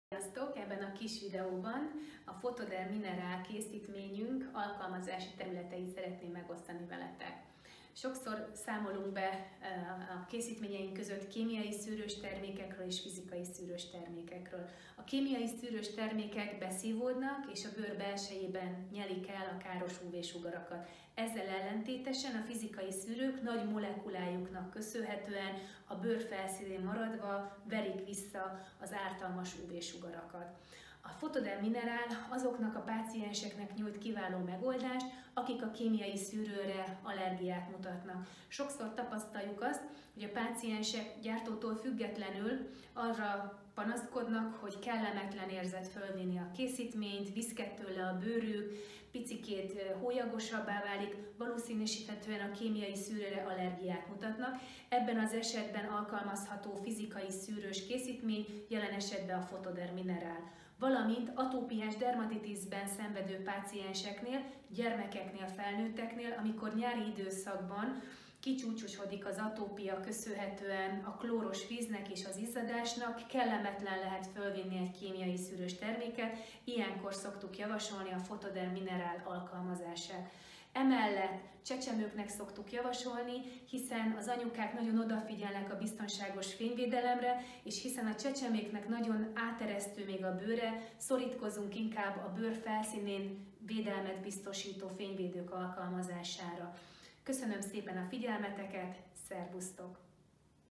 Mi a különbség a kémiai és fizikai fényszűrők között?
Mikor válasszuk a fizikai fényszűrőt tartalmazó Bioderma Photoderm Mineral fényvédő sprayt?